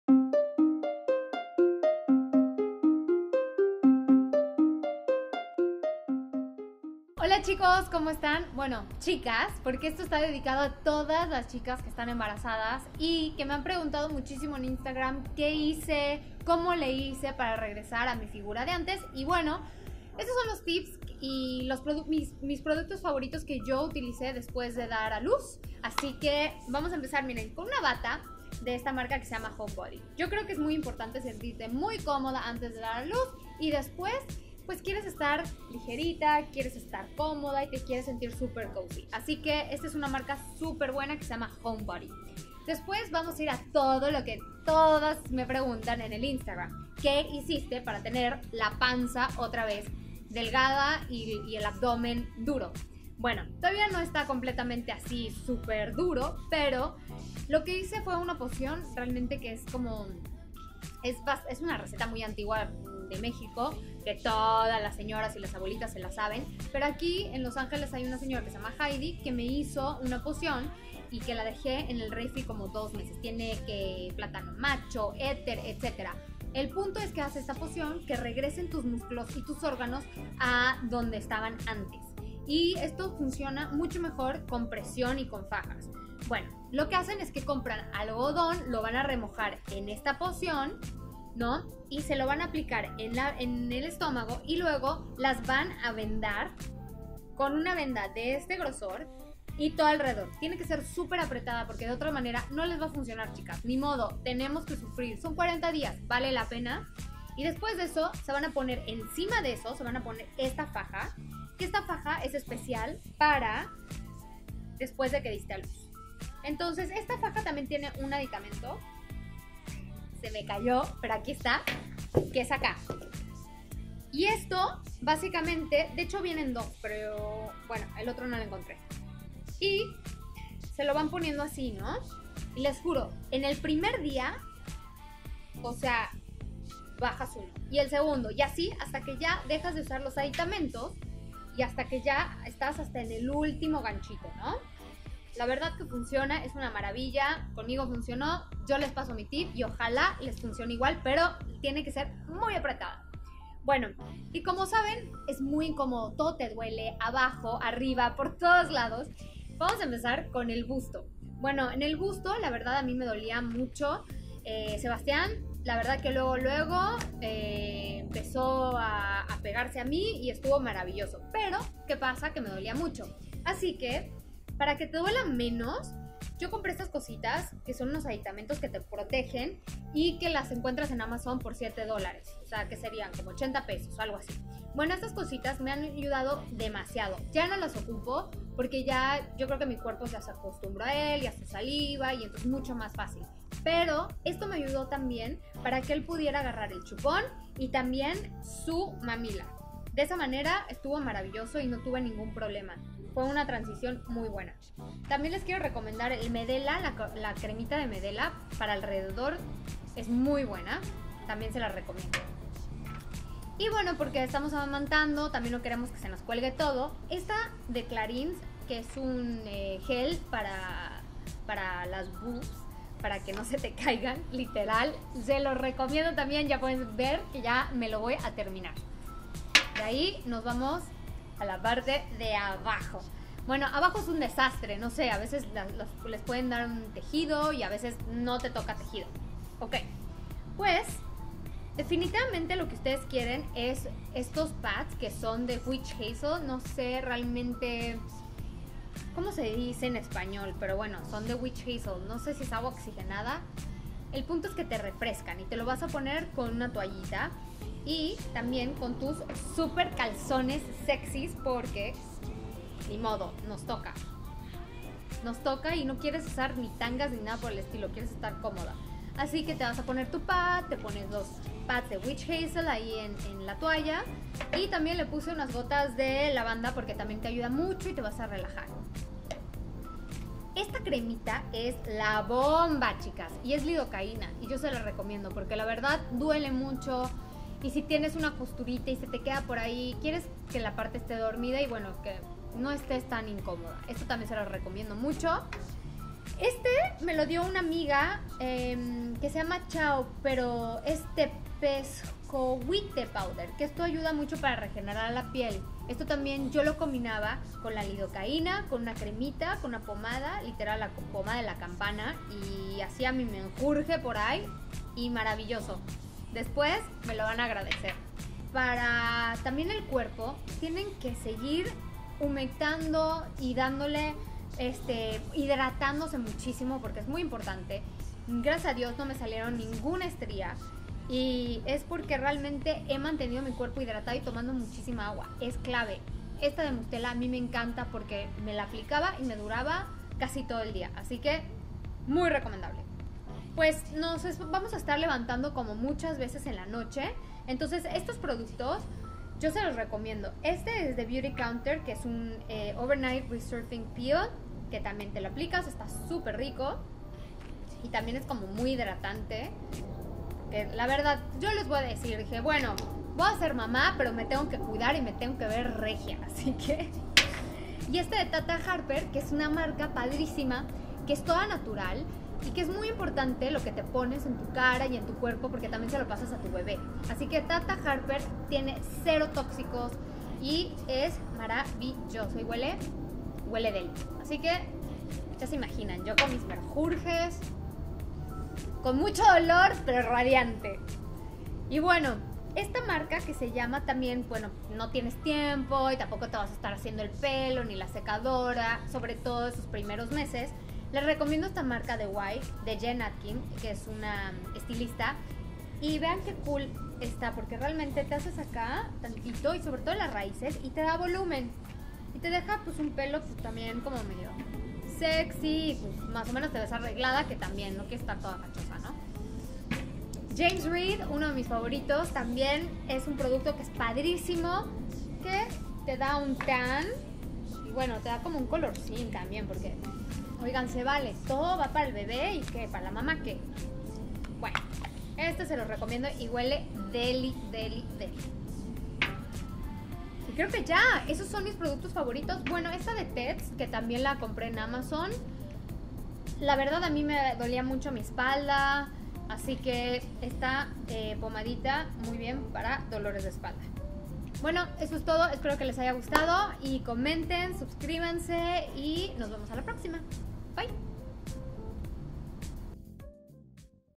MIS PRODUCTOS FAVORITOS PARA EL POSTPARTO /FAJAS-GEL REDUCTOR Y ME COMI MI PLACENTA!/FERNANDA ROMERO
FAJAS Y CUIDADOS POSTPARTO - RECUPERA TU FIGURA!! / MIS CONSEJOS

Bueno pues se que me tarde bastante en hacer este video pero ya me tocaba compartirlo.
La realidad del postaprto y las cosas wue no nos cuentan. pero aqui les tengo los productos necesarios.
Muchas de ustedes siempre me han preguntado que hice para regresar a mi figura de antes y que hice para el dolor etc..
Te doy el review de la faja  que sirve excelente para después de tener un bebé, ya sea por via natural o cesárea  :)
Recuerda, no te desesperes por recuperar tu figura, antes que nada cuida tu salud, pero si te ayudas de alguna de estas fajas, seguro te sientes mejor y la pérdida de tallas llegará más pronto.
Bueno aquí les va la lista de mis productos favoritos y los que hicieron mi vida mucho mas fácil  durante y después del embarazo.

La mayoría de ellos son orgánicos y hasta algunos pueden ser hechos en casa.

Aquí les van los links también

Maderoterapia @maderoterapia ella esta aqui en Los Angeles California

Bronceador James Read

Bueno Mamis gracias por verme y les deseo una pronta recupracion.